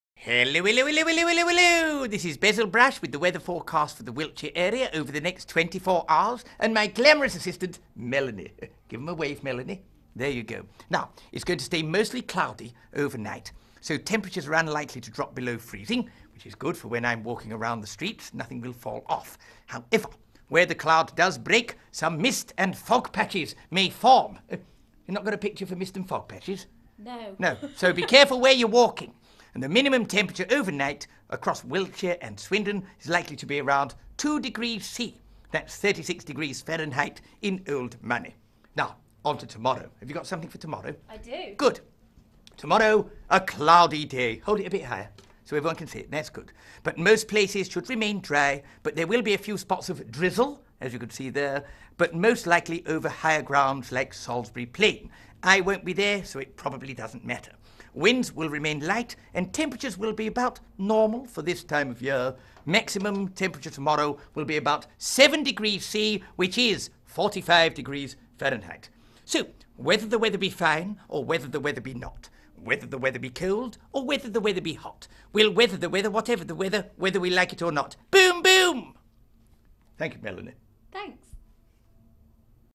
Basil Brush has today's weather forecast
Basil Brush visited BBC Wiltshire on the 10th of February and offered to read this evening's weather forecast!